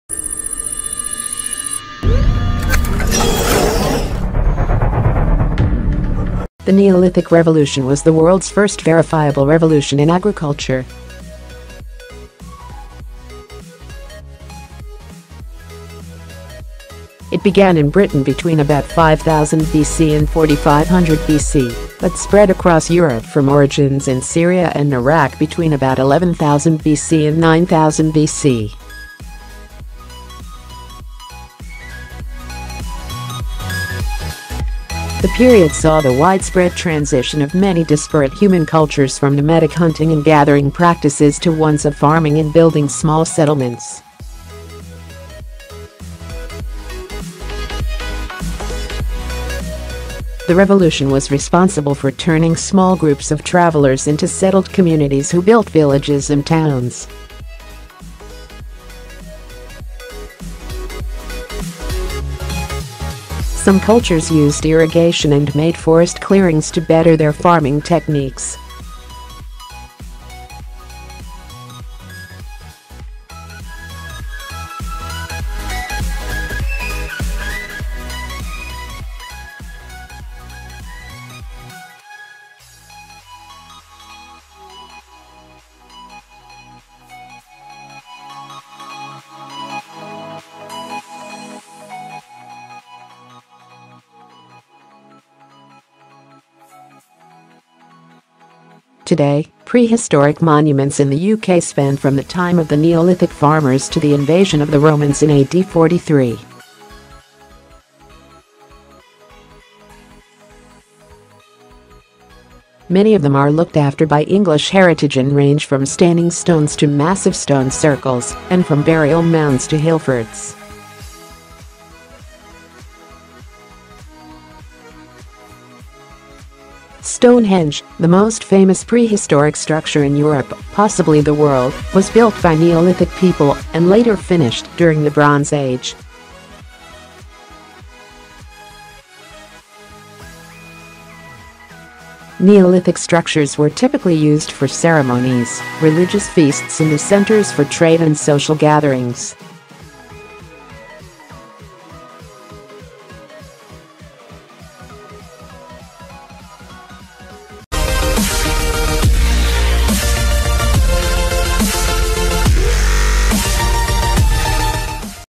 WHAT DO WE KNOW ABOUT NEOLITHIC BRITAIN? - News Techcology
The Neolithic Revolution was the world's first verifiable revolution in agriculture.It began in Britain between about 5000 BC and 4500 BC but spread across Europe from origins in Syria and Iraq between about 11000 BC and 9000 BC.The period saw the widespread transition of many disparate human cultures from nomadic hunting and gathering practices to ones of farming and building small settlements.The revolution was responsible for turning small groups of travellers into settled communities who built villages and towns.Some cultures used irrigation and made forest clearings to better their farming techniques.Others stored food for times of hunger, and farming eventually created different roles and divisions of labour in societies as well as trading economies.In the UK, the period was triggered by a huge migration or folk-movement from across the Channel.Today, prehistoric monuments in the UK span from the time of the Neolithic farmers to the invasion of the Romans in AD 43.Many of them are looked after by English Heritage and range from standing stones to massive stone circles, and from burial mounds to hillforts.Stonehenge, the most famous prehistoric structure in Europe, possibly the world, was built by Neolithic people, and later finished during the Bronze Age.Neolithic structures were typically used for ceremonies, religious feasts and as centres for trade and social gatherings.